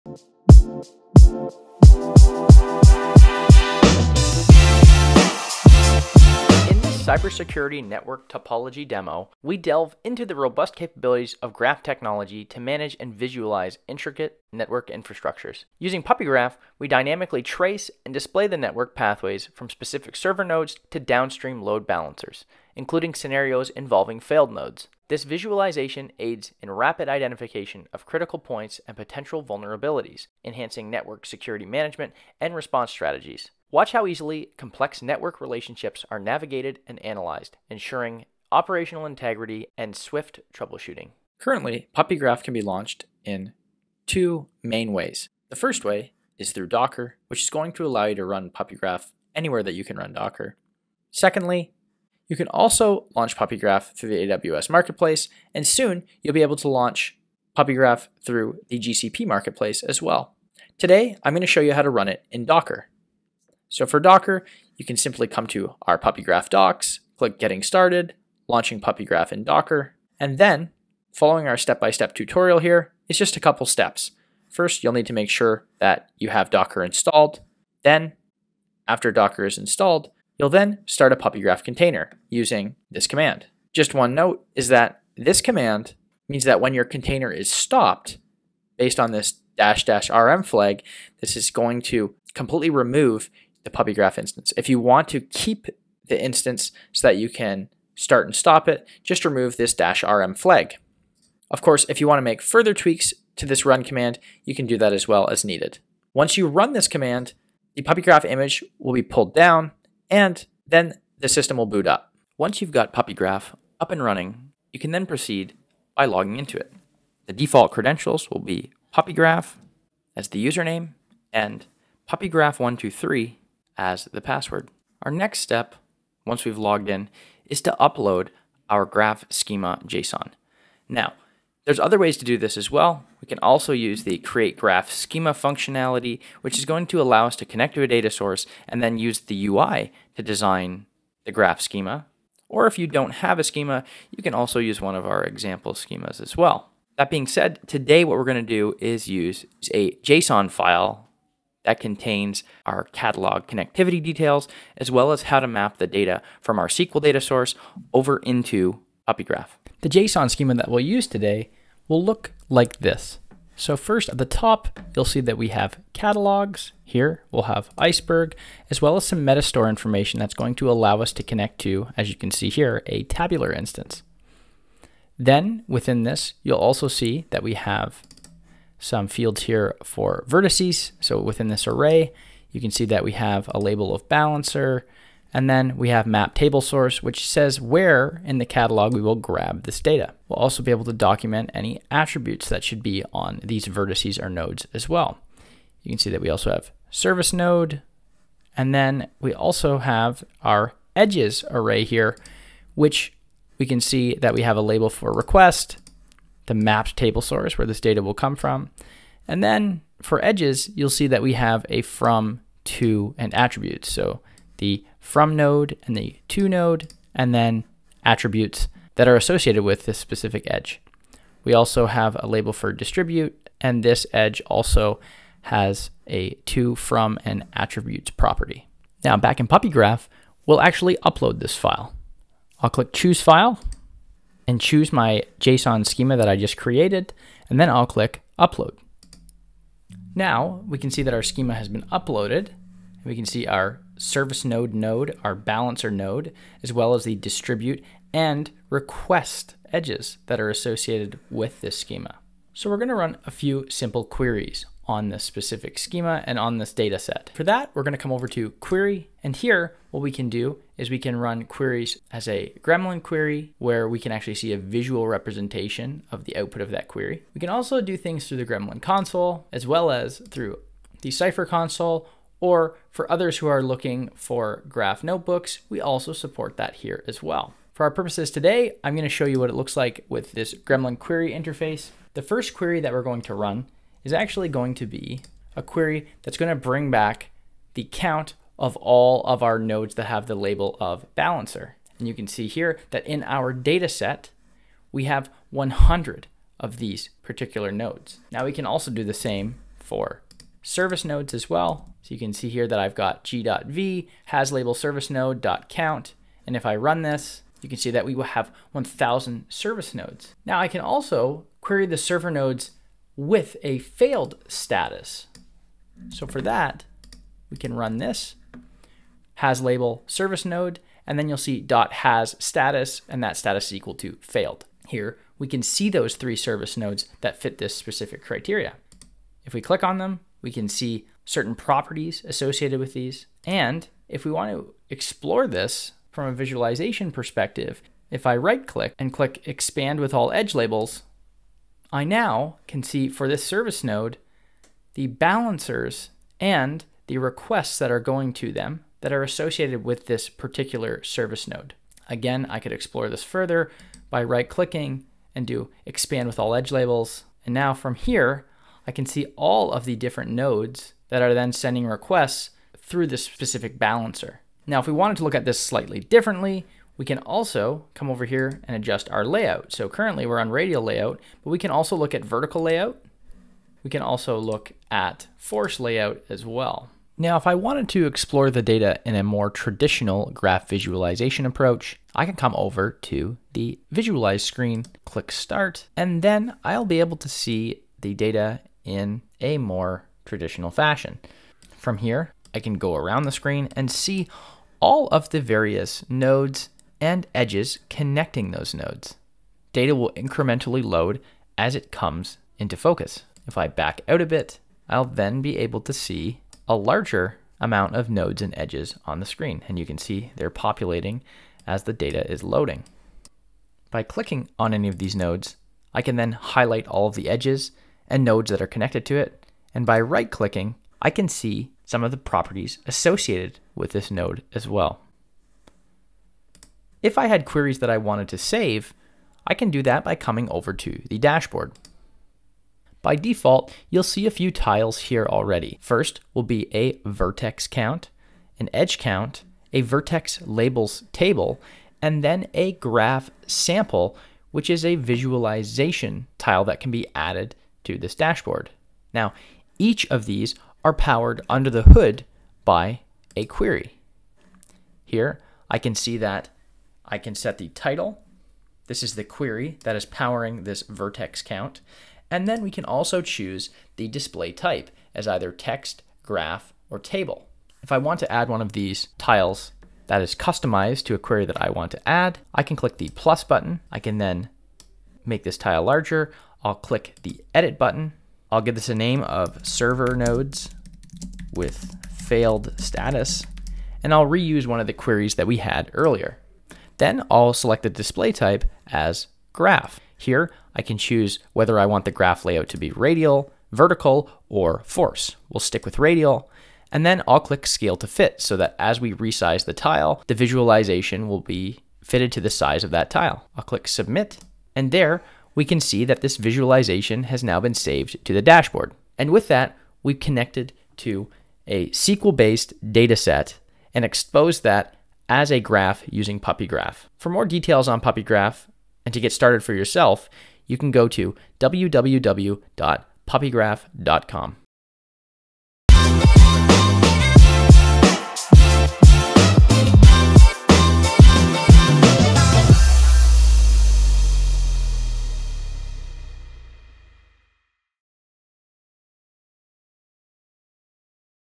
Network Topology Demo
In our Cybersecurity Network Topology demo, we delve into the robust capabilities of graph technology to manage and visualize intricate network infrastructures. Using PuppyGraph, we dynamically trace and display the network pathways from specific server nodes to downstream load balancers, including scenarios involving failed nodes. This visualization aids in rapid identification of critical points and potential vulnerabilities, enhancing network security management and response strategies. Watch how easily complex network relationships are navigated and analyzed, ensuring operational integrity and swift troubleshooting.